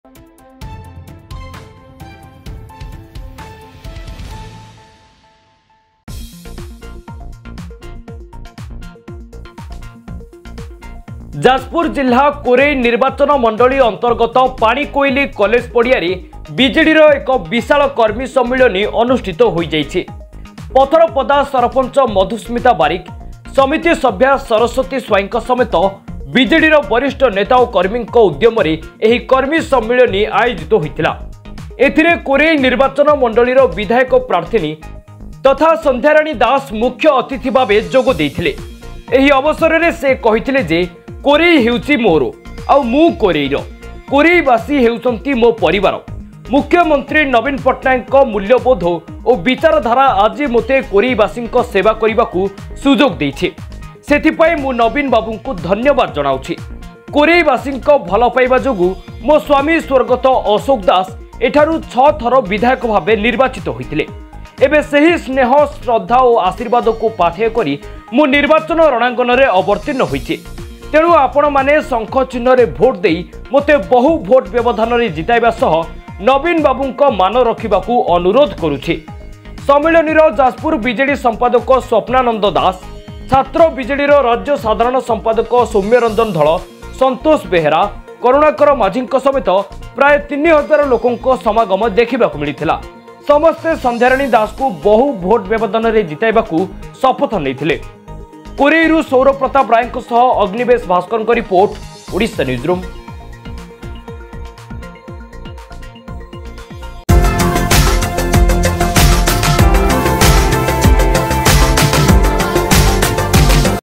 ପଥରପଦାରେ ବିଜେଡିର କର୍ମୀ ସମ୍ମିଳନୀ, ସଂଧ୍ୟାରାଣୀ ଦାସଙ୍କୁ ଜିତାଇବାକୁ ଶପଥ | Odisha Newsroom
ପଥରପଦାରେ ବିଜେଡିର କର୍ମୀ ସମ୍ମିଳନୀ, ସଂଧ୍ୟାରାଣୀ ଦାସଙ୍କ ଜିତାଇବାକୁ ଶପଥ